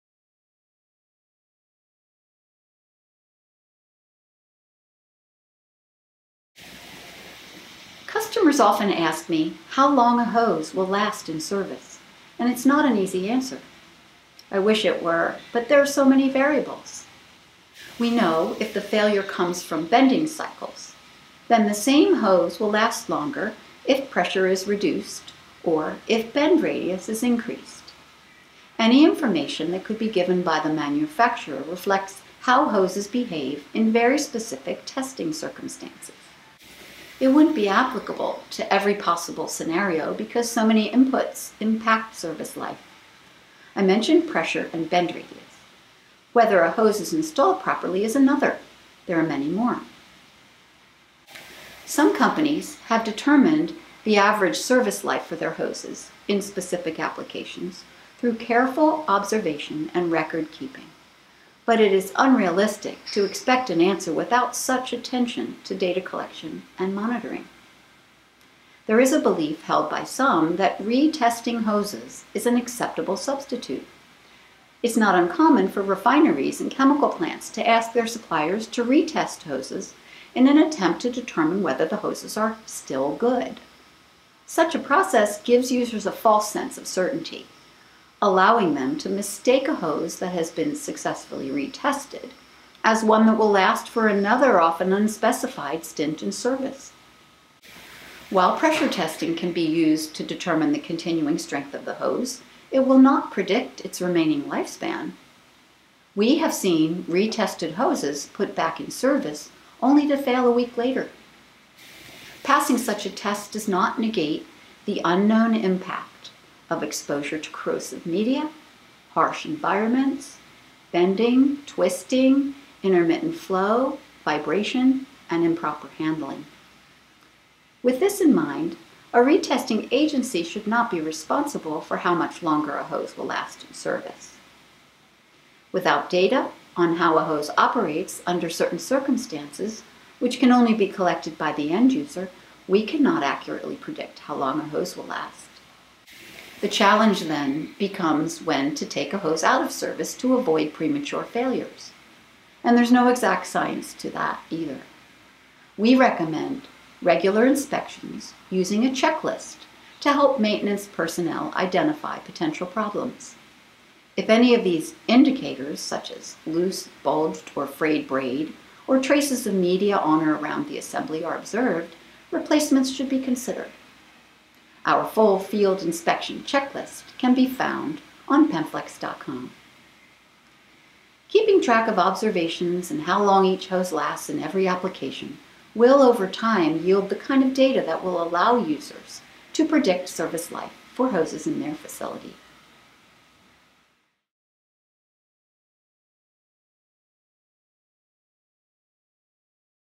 How Long Will a Metal Hose Last in Service?
Knowing how long a metal hose will last in service would make life easier. We could more accurately plan purchases for replacement parts and then schedule time to install those parts, all while reducing the likelihood of failures.

While this isn’t a pipe dream—some companies have successfully determined average service life for hoses in specific applications through careful observation and record-keeping—it is unrealistic to expect that an answer can be given without such attention to data collection and the monitoring of outcomes.

This video accompanies an Engineering Bulletin on Penflex.com. To learn more, have a look